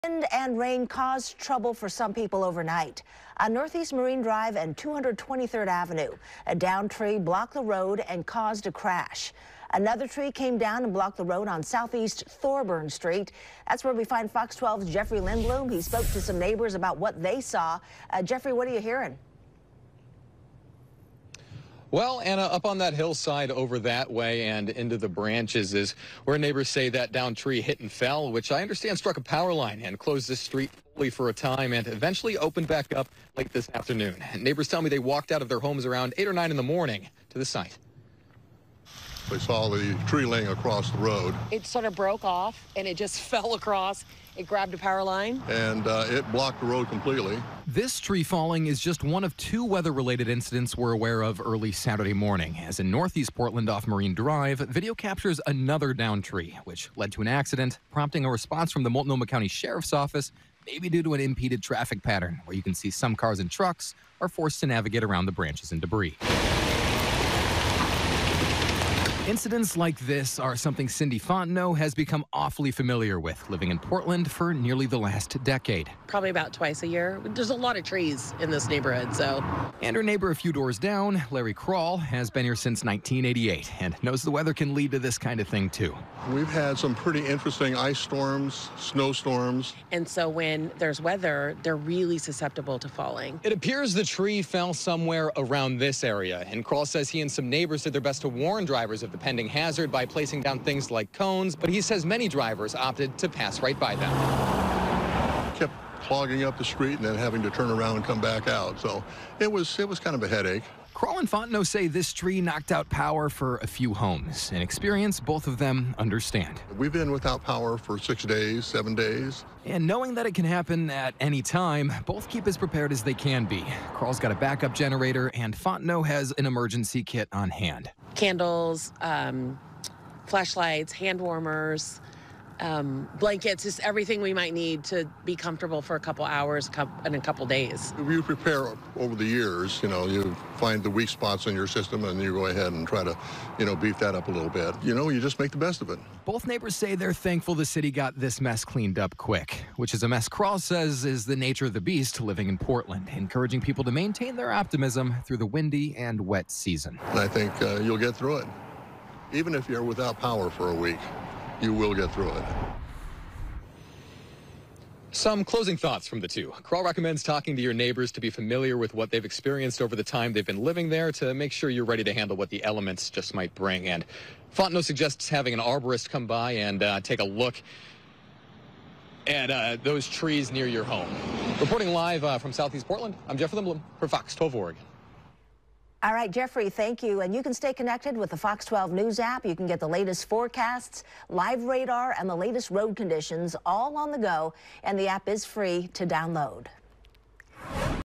Portlanders wake to downed trees, power outages
People in the Portland area woke up to downed trees and power outages Saturday morning.